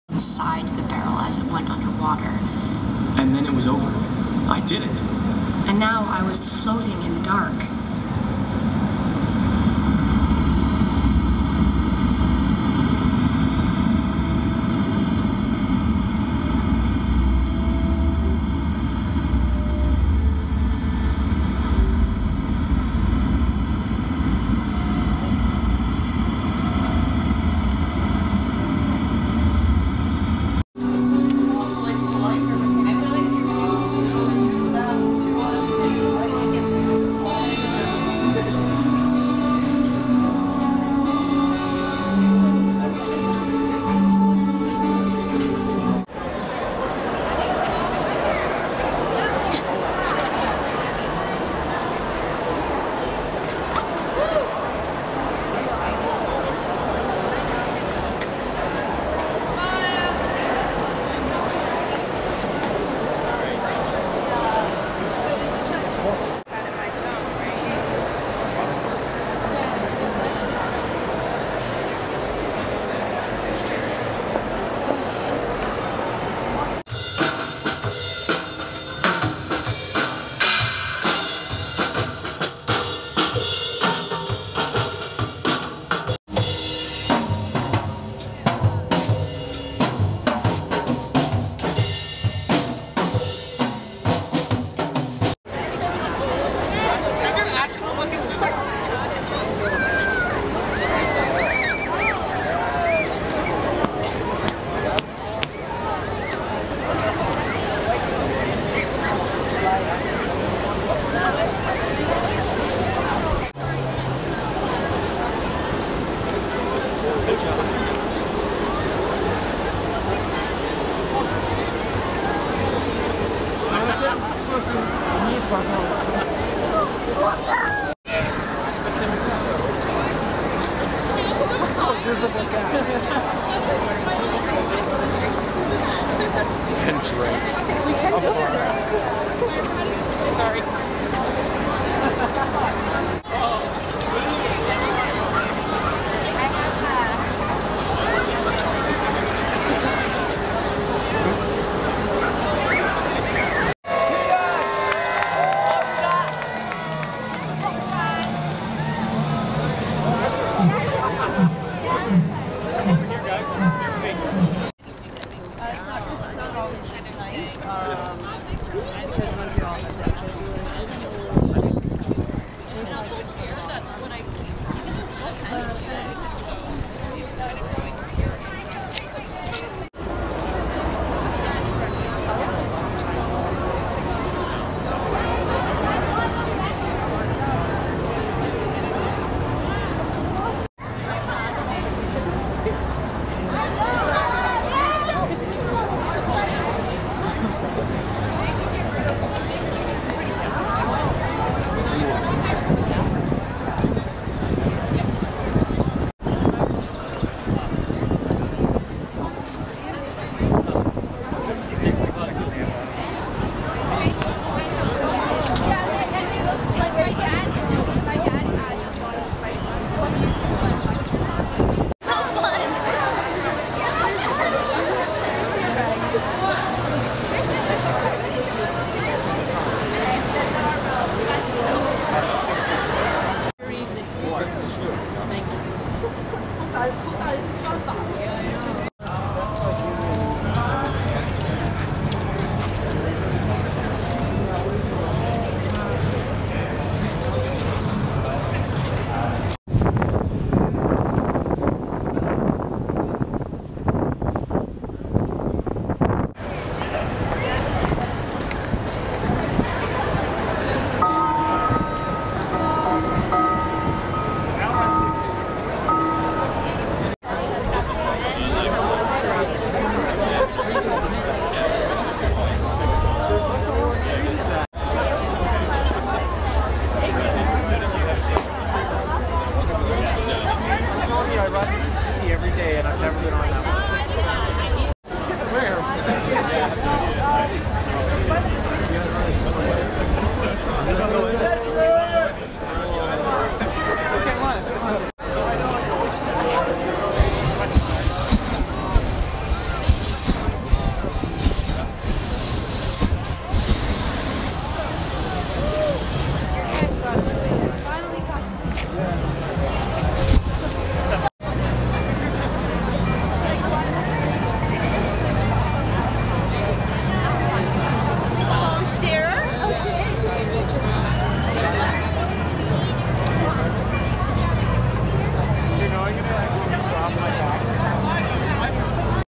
Nuit Blanche Toronto 2011 Part 2
6th annual Nuit Blanche was a cold, windy night.